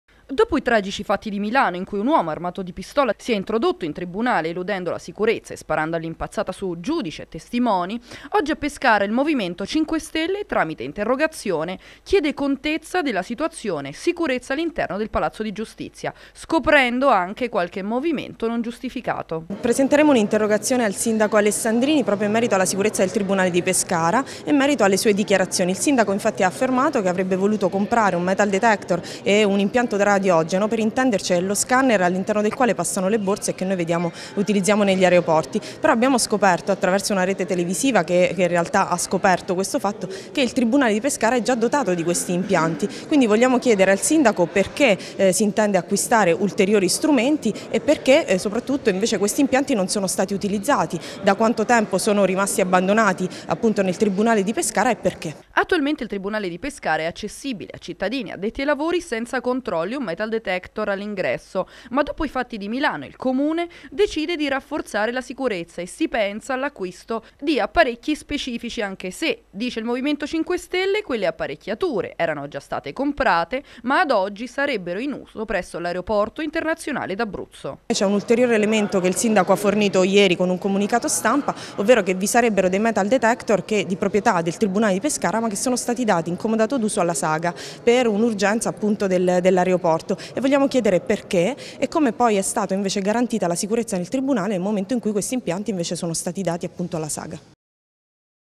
Sabatini. Sicurezza in tribunale
Il m5s fa un interpellanza a Pescara per chiedere dettagli in merito alla sicurezza alla interno del palazzo di giustizia. Secondo le dichiarazioni dei Grillini, il comune, avrebbe dato in uso alla aeroporto d'Abruzzo la strumentazione destinata al tribunale.